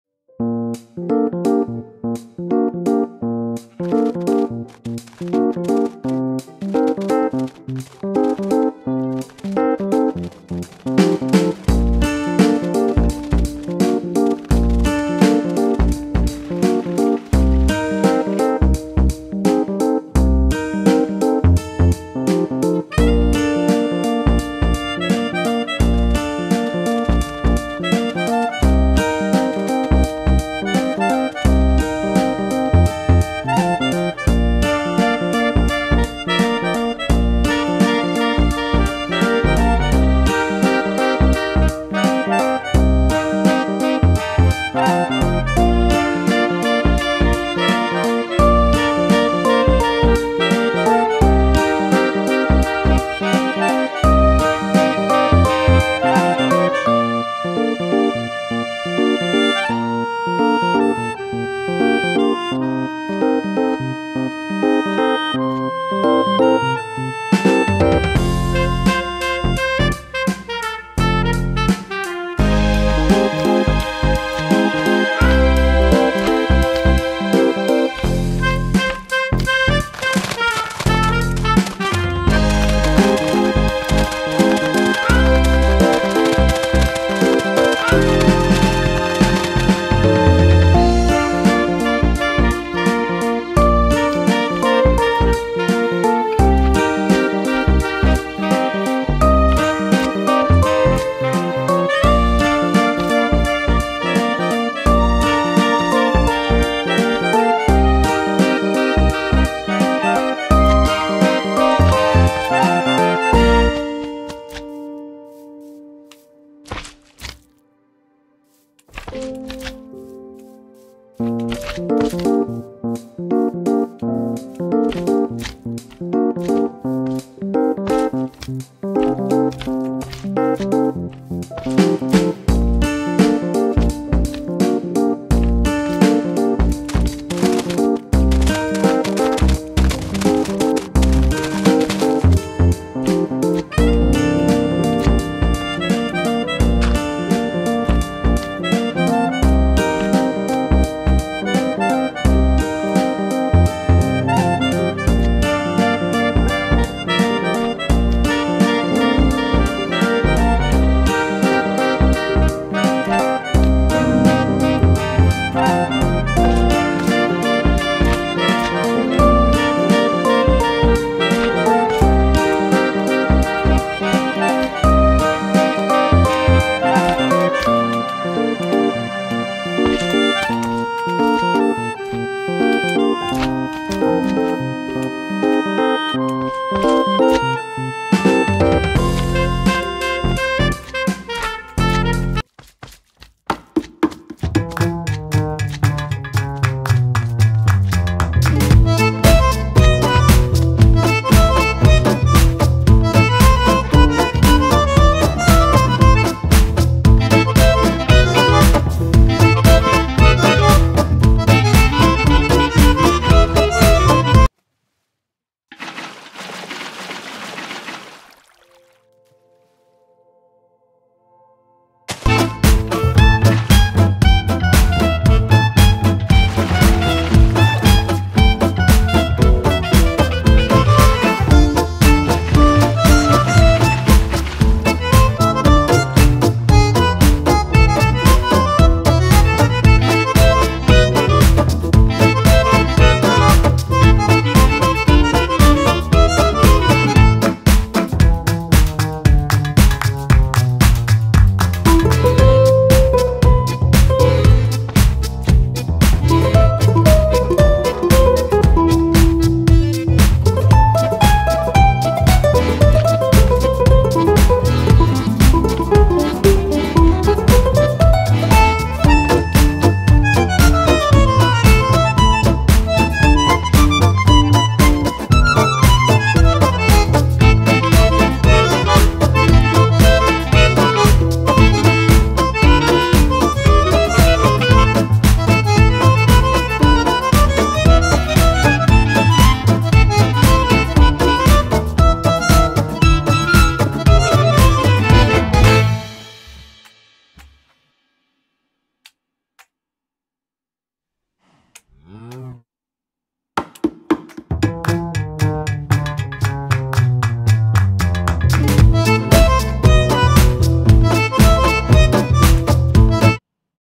Water Physics .vs. Physics Mod
Who will win the two rounds, Water Tower and House Flood